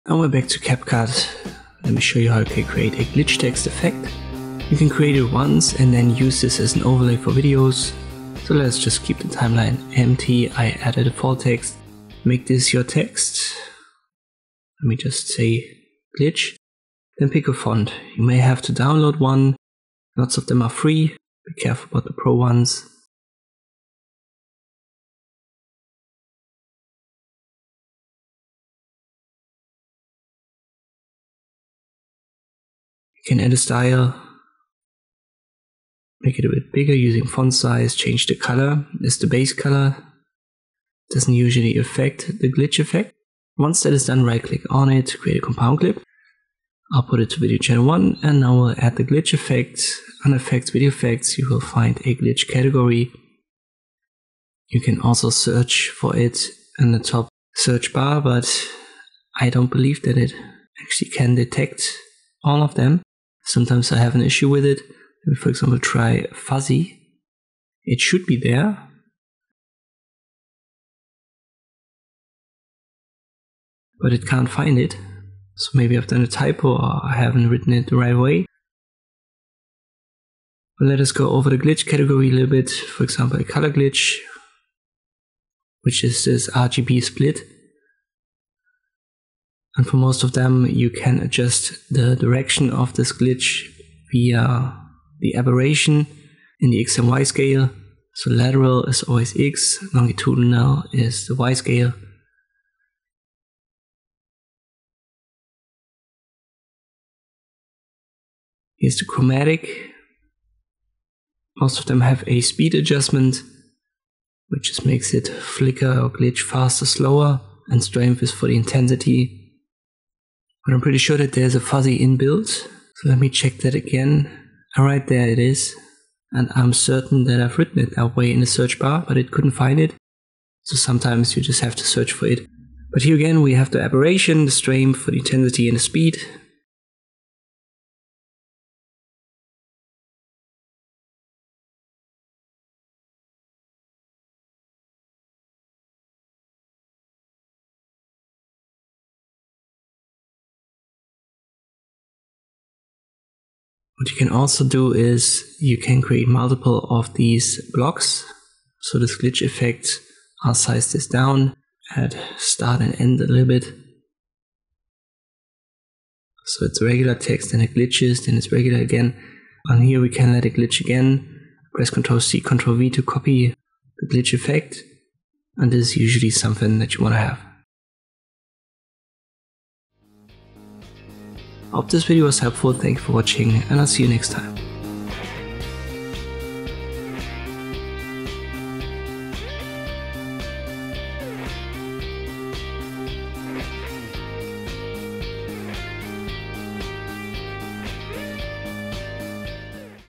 How to Create a Text Glitch Effect in CapCut
In this tutorial I'll show you how to add a glitch text effect to videos in CapCut.